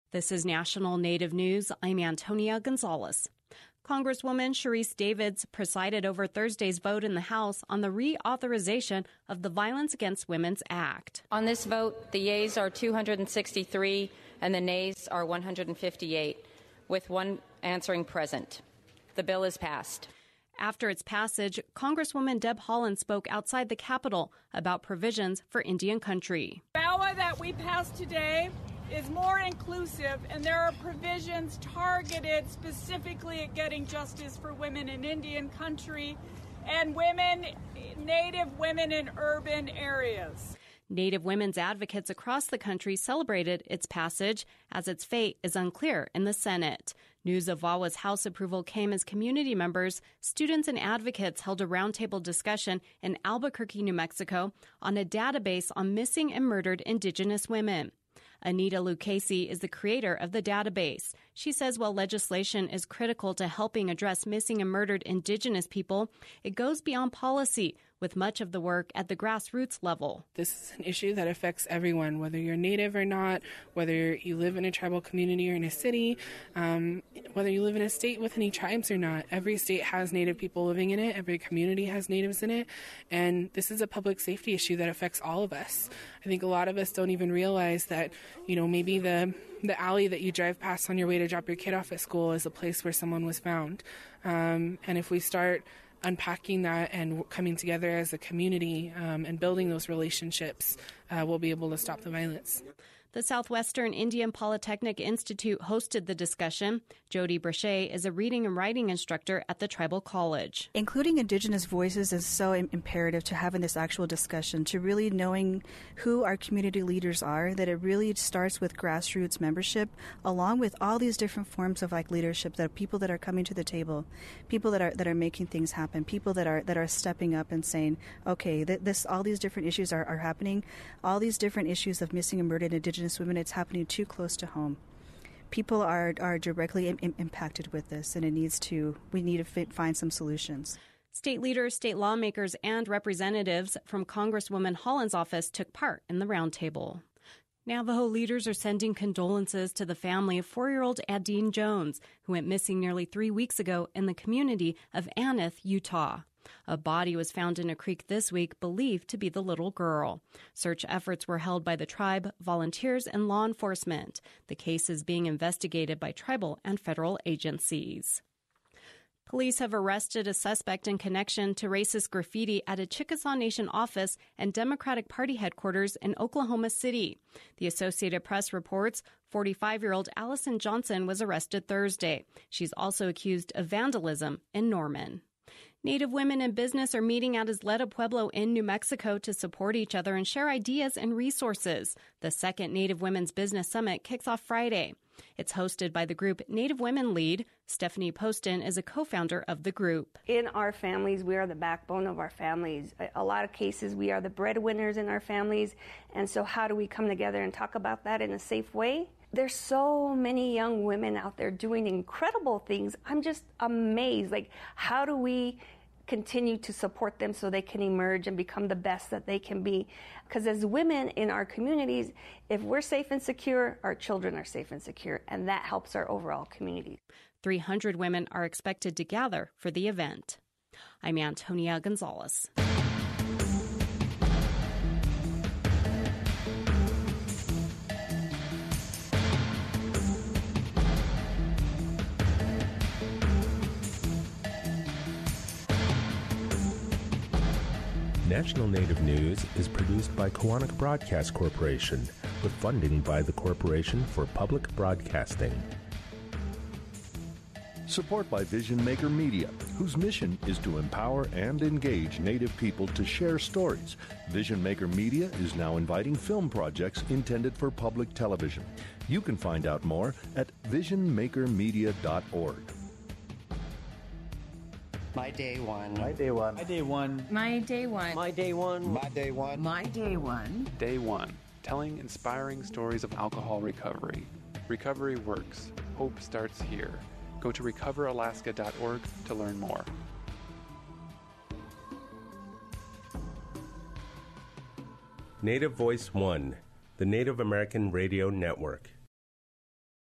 Friday, April 5, 2019
Congresswoman Sharice Davids presides over House vote on VAWA

Tribal college hosts missing and murdered Indigenous women roundtable

Native Women Lead host second Native Women Business summit